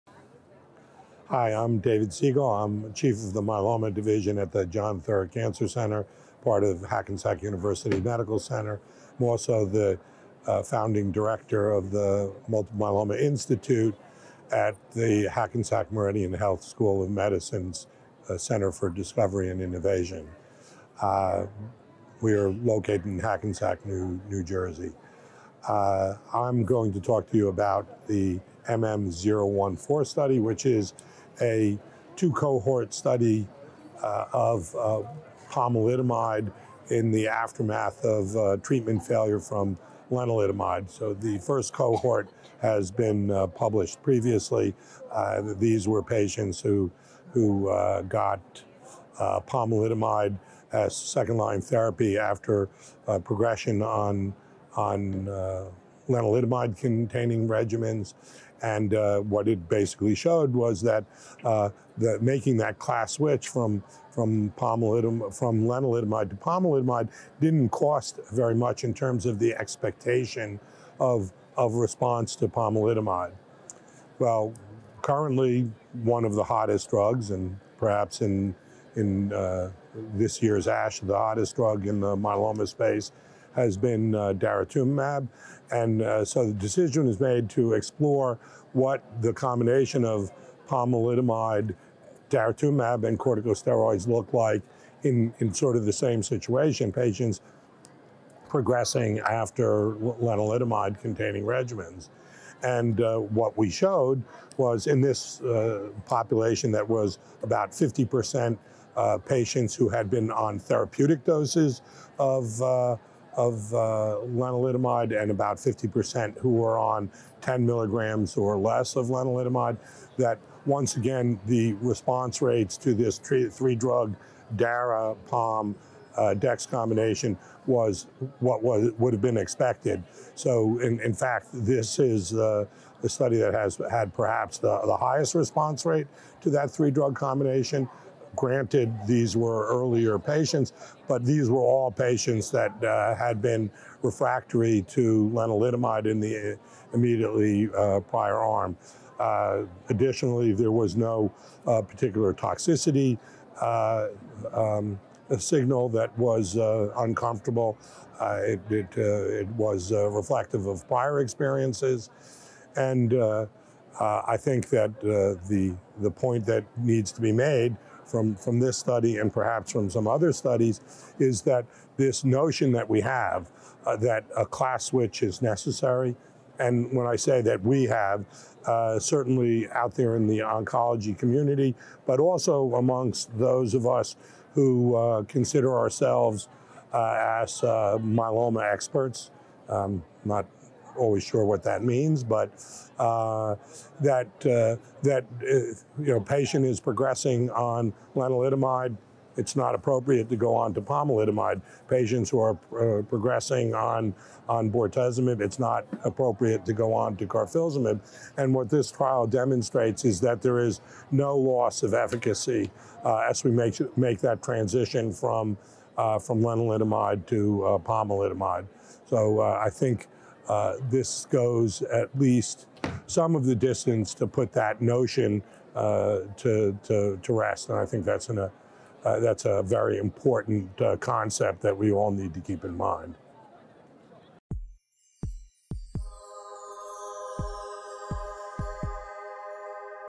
Pomalidomide + Low-Dose Dex + Dara in Relapsed and/or Refractory Multiple Myeloma
Lenalidomide (LEN) until progressive disease (PD) is an established treatment (Tx) in newly diagnosed and relapsed and/or refractory multiple myeloma (RRMM); thus, patients (pts) for whom the benefit of LEN has been exhausted are a clinically relevant population. However, LEN-refractory pts have typically been excluded from recent clinical trials investigating triplet regimens after ≤ 3 prior Tx lines. MM-014 (NCT01946477) is an ongoing phase 2 study that was designed to assess the safety and efficacy of pomalidomide (POM)-based Tx regimens in pts with RRMM and first- or second-line LEN Tx failure immediately before study entry. Earlier results from cohort A (POM + low-dose dexamethasone [LoDEX]) and cohort B (POM + LoDEX + daratumumab [DARA]) indicate that POM-based Tx is safe and effective in this setting. Here we report updated results from cohort B.

LEN-refractory pts with RRMM are in need of effective Tx options. MM-014 is the first prospective clinical trial to investigate a POM-based doublet or triplet regimen immediately after LEN-based Tx failure. In the context of a relatively short follow-up, the 9-mo PFS rate (86.3%) is promising. The ORR (77.7%) was higher than that previously reported with this triplet combination in heavily pre-treated pts with RRMM (≥ 2 prior lines [median, 4]; ORR, 60%), and the rate of grade 3/4 neutropenia in the present study was lower (61.6% vs 77%). These updated results from cohort B continue to demonstrate that POM + LoDEX + DARA is safe and effective following first- or second-line LEN-based Tx failure and further support earlier use of POM-based Tx in pts with RRMM

Category
Nonprofits & Activism
License